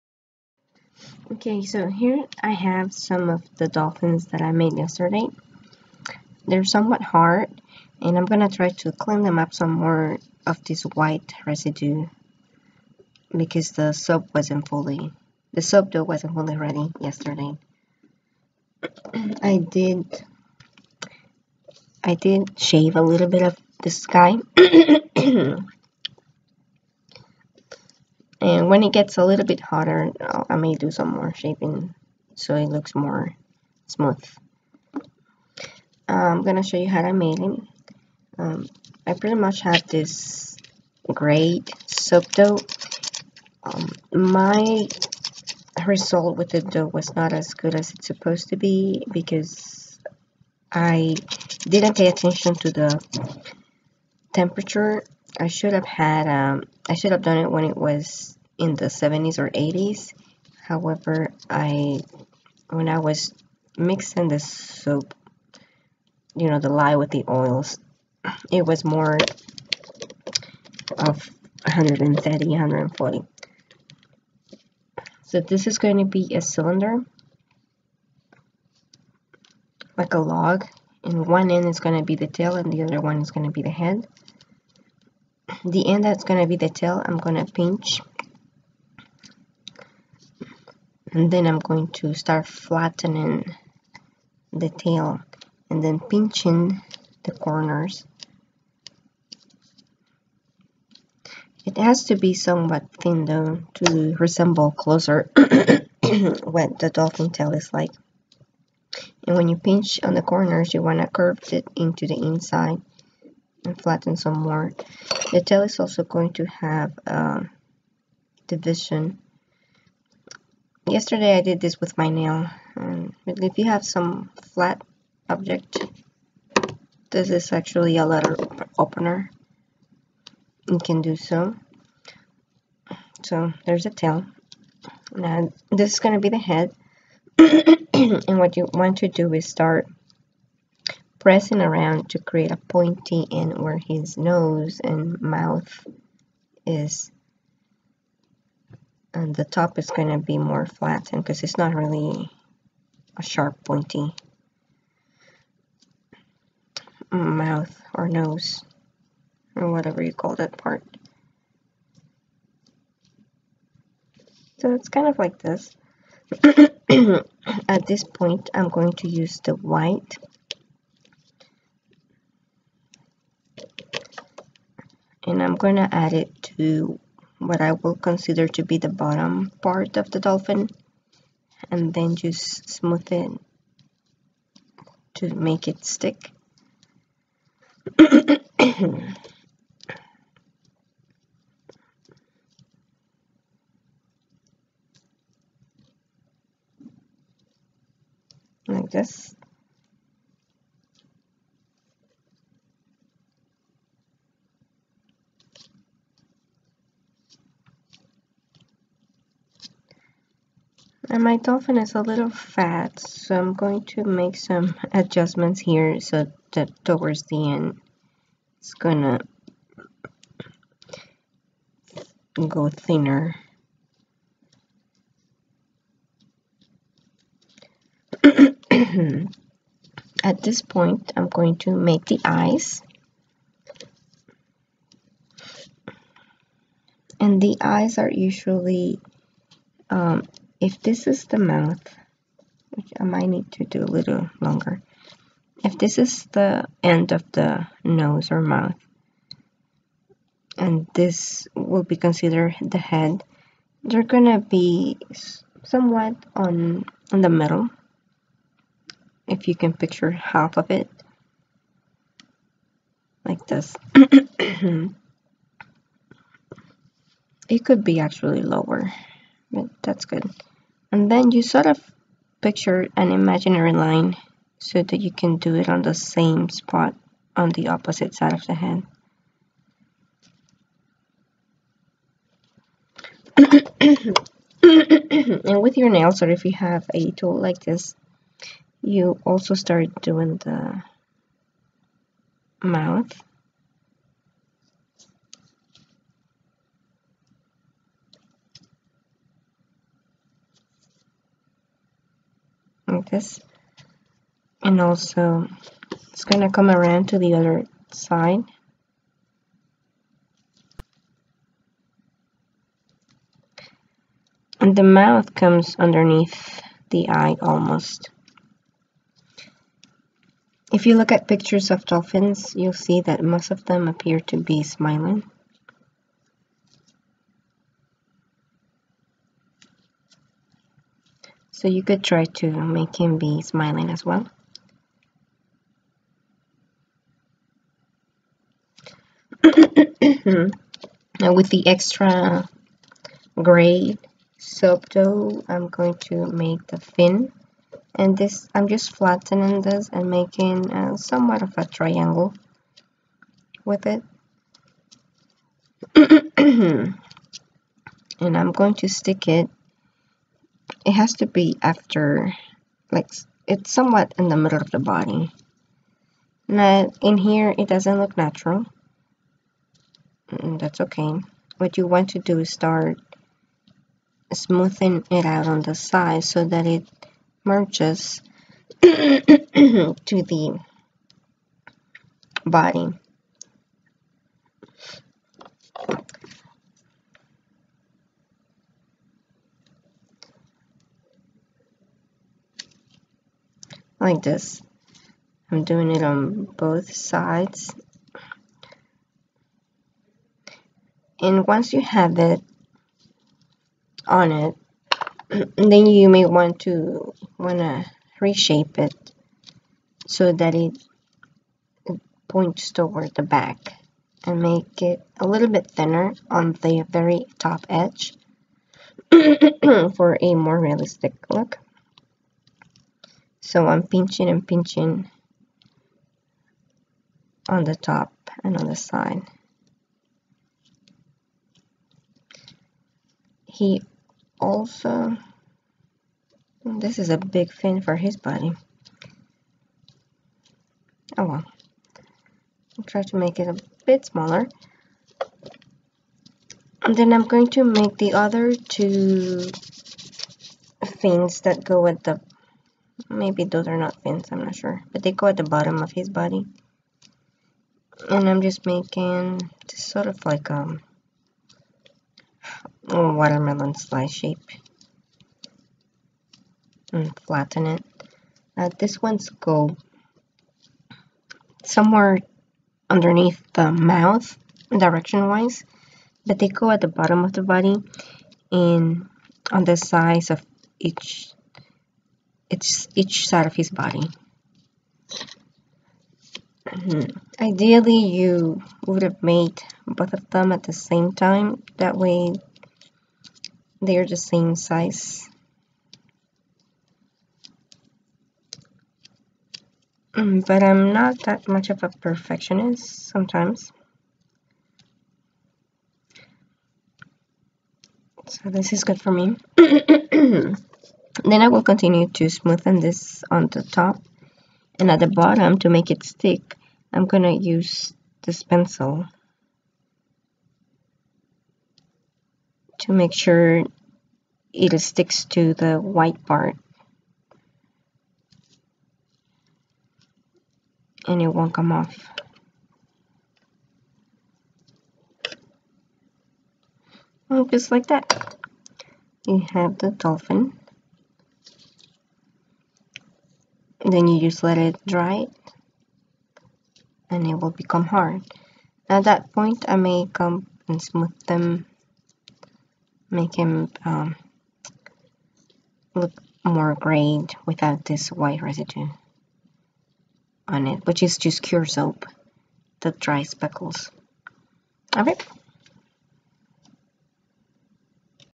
How to sculpt dolphin out of soap dough
Making dolphins embeds with cold process soap dough. The shaping principles could be applied to polymer clay too.